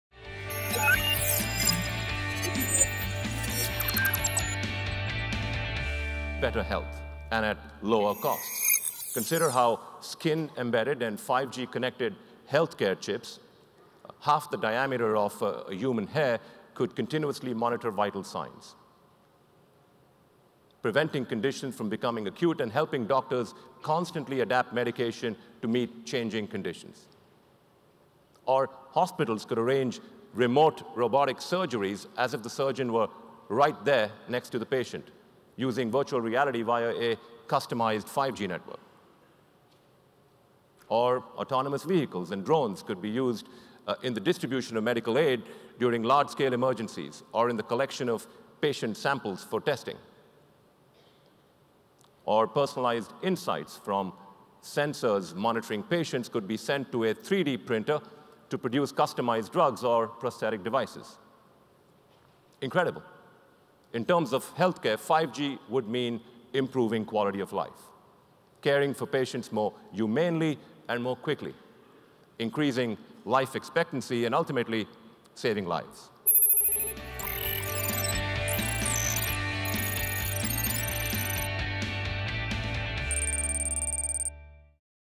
Keynote Byte: Nokia Suri on 5G & Healthcare
During his keynote on Day 2 of SuperMobility, Nokia President & CEO Rajeev Suri discussed the benefits that 5G will have on the healthcare industry and thus, improving the quality of life.